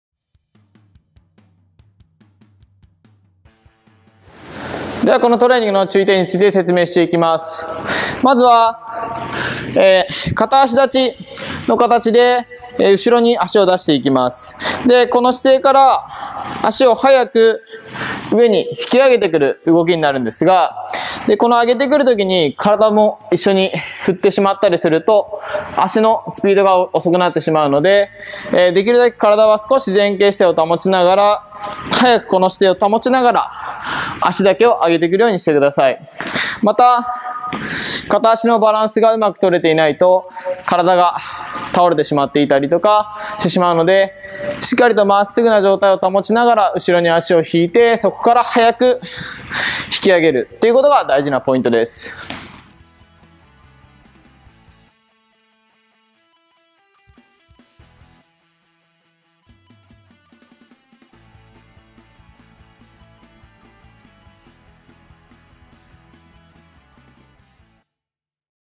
バランス能力と走力アップトレーニング
【目的/上達スキル】
バランス能力を獲得するため
瞬発的な力の発揮の獲得のため
足の回転数を上げ走力アップにつなげるため

【目安所要時間】
3分程度(左右各10回×1〜3セット)

【手順】
1.片足でバランスをとる
　(少し上半身は前傾姿勢)
2.浮かせている足を後方に引き、後ろに引いた側の腕を前に出すようにする
　(走る時の姿勢を作る)
3.後ろに引いた足を素早く前に引き上げる
　(バランスが崩れないようにする)
4.再度引き上げた足を後ろに引き続けて行う

【指導のコツ】
・上半身を少し前傾させることで足を引き上げた時に体がぶれにくくなる
・腕の振りも使うことでバランスも取りやすくなる

【指導のポイント】
・足を引き上げていく時に上半身は動かないようにすること
・片足でのバランスをとった状態で足をあげること
・腕も足の動きに合わせて振ること

野球専門・動作解析サポート
BASEBALL ONE